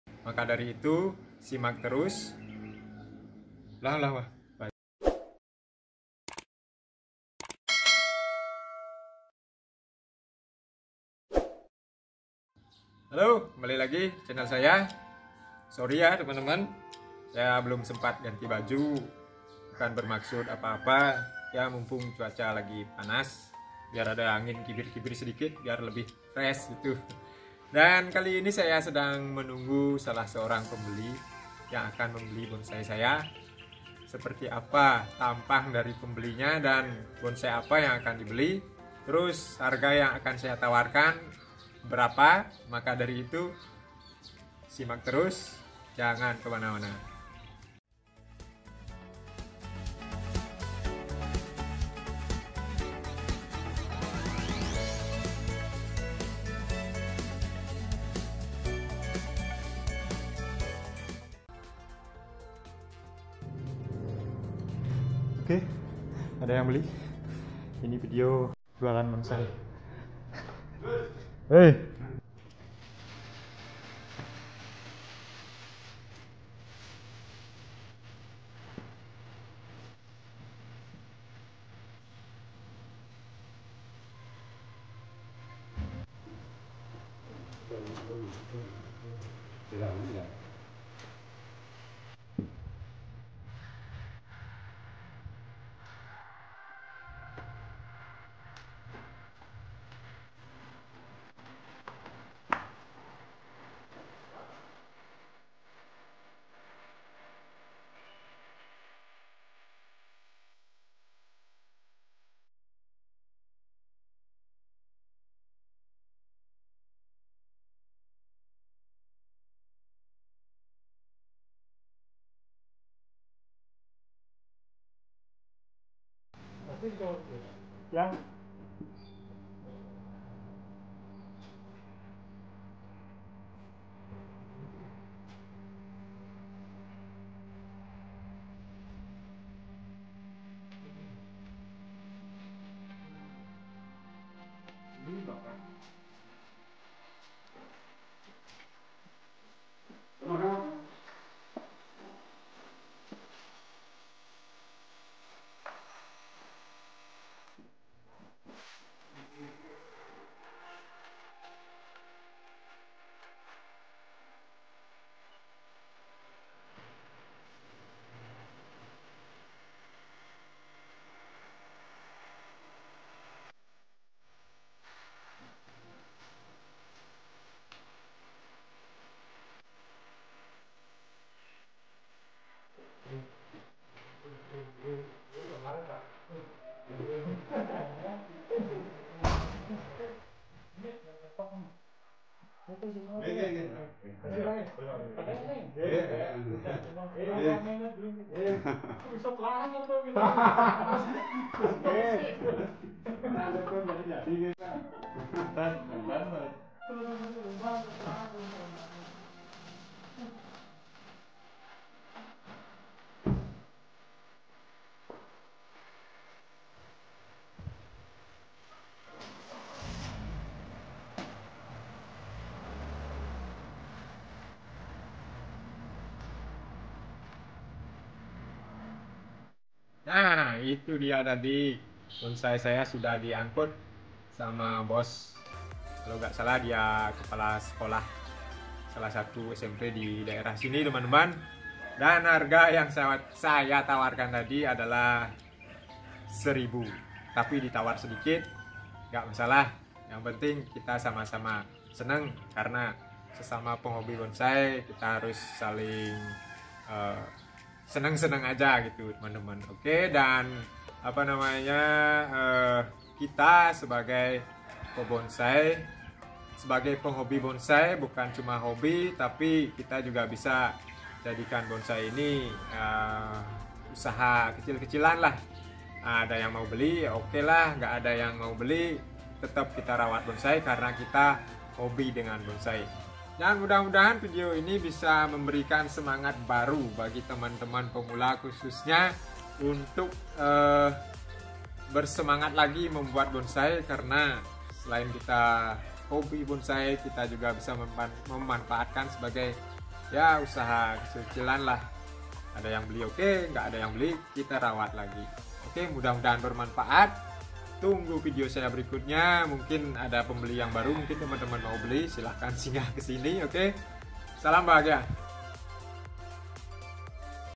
Jualan Bonsai Arabica
halo para pemula bonsai, kali ini adalah video jualan bonsai dari saya dan bonsai yang terjual adalah bonsai arabica. tujuan saya membuat video ini adalah untuk memberikan inspirasi dan semangat bagi teman teman pemula agar lebih bersemangat lagi untuk membuat bonsai.